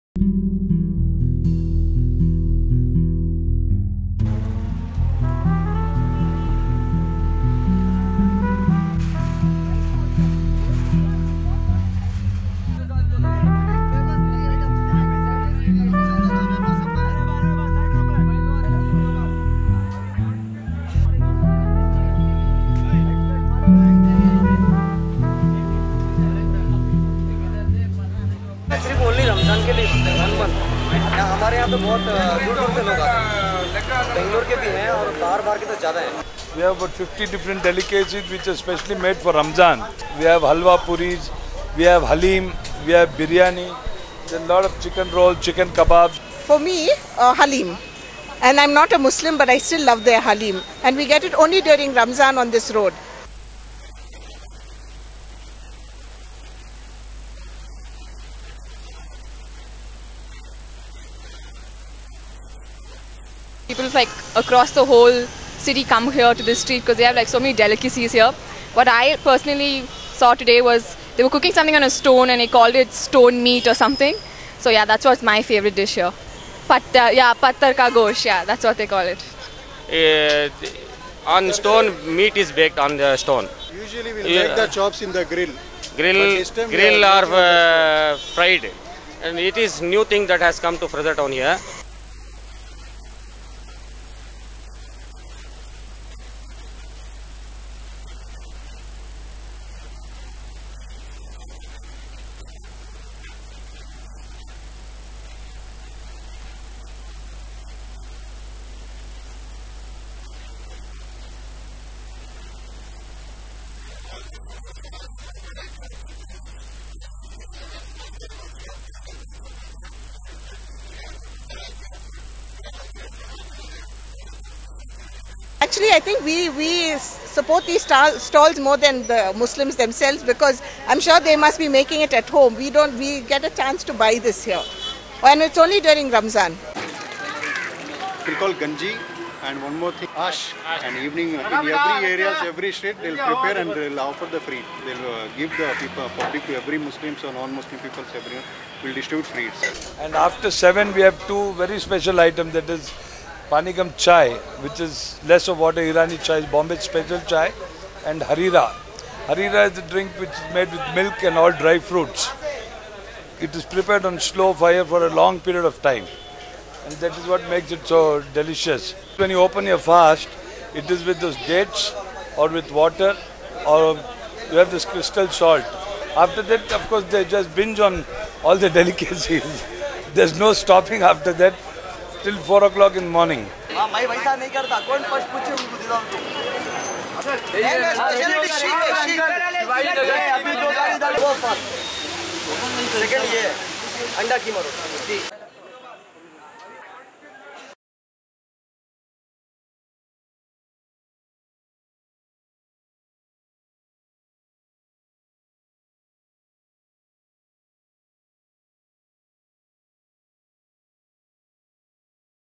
Fasting and feasting
Mosque Road (M.M. Road) in Fraser Town, Bangalore, turns into Foodie Paradise during the holy month of Ramzan. Traditional Ramzan dishes, some of which take hours of preparation, are served till midnight and people from all cultures come here to taste the delicacies.